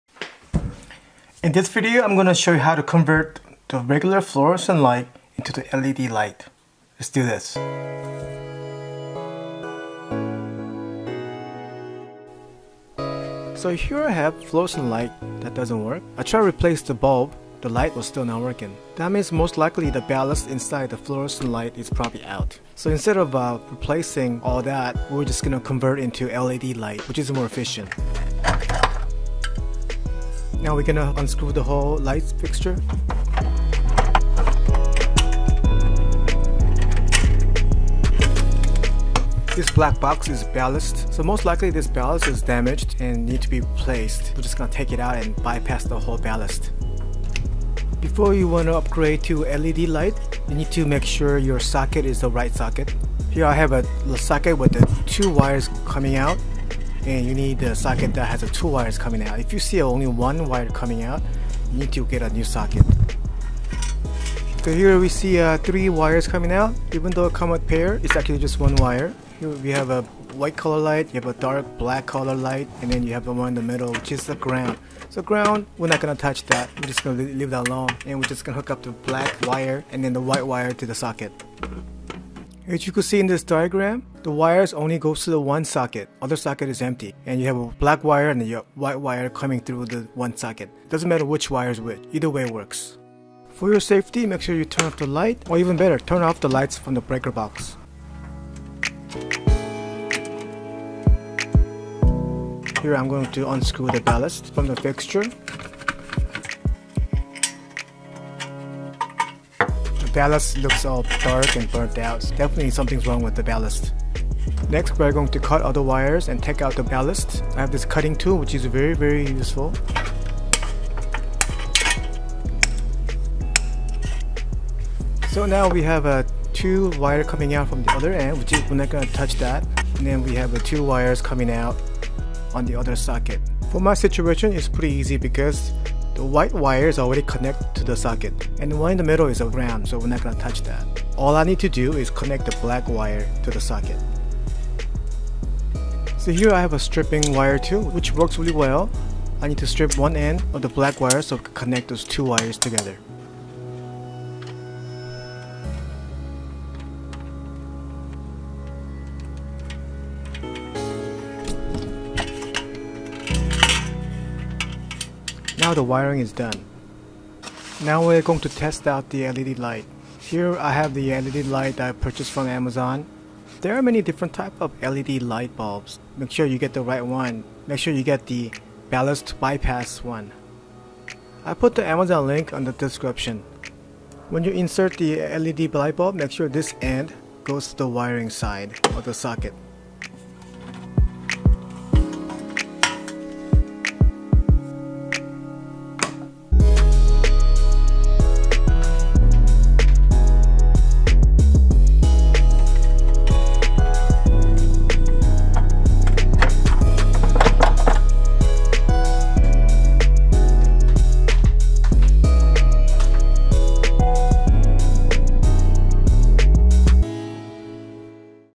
How to Fix Your Fluorescent Light by Upgrading to LED Light
2ft LED light bulb
Wire Stripper Tool
Wire Cutting Tool
Wire Connector Tool

In this video, I am going to fix fluorescent light by upgrading to LED.  I have broken fluorescent light in the kitchen.  I tried to fix it by replace the blub but it didn't help.  So most likely, the ballast inside the fluorescent light fixture needs to be replace.  But instead of replacing ballast, I decided to bypass ballast and upgrade to LED light bulb which is more efficient.